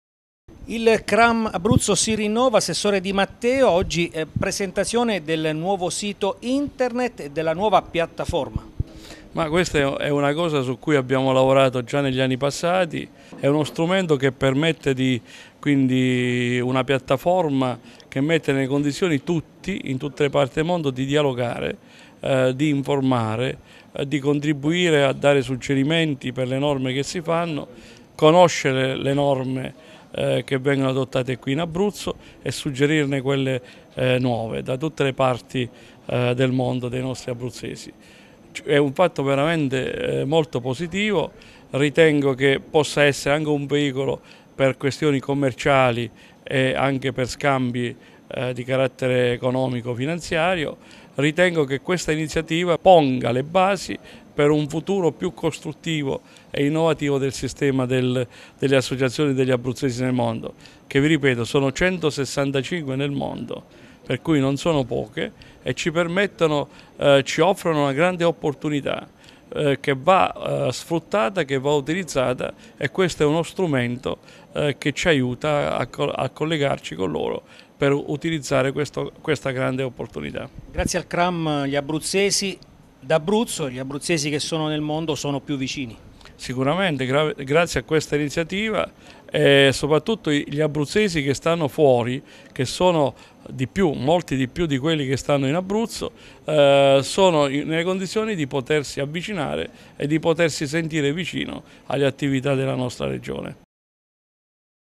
Il nuovo sito internet del Cram Abruzzo
Presentato a Pescara il sito internet del Cram (Consiglio regionale abruzzesi nel mondo) e la piattaforma virtuale Piazza Abruzzo, per condividere opportunità per gli abruzzesi che vogliono andare all’estero e per gli abruzzesi nel mondo che vogliono restare in contatto con la loro terra d’origine.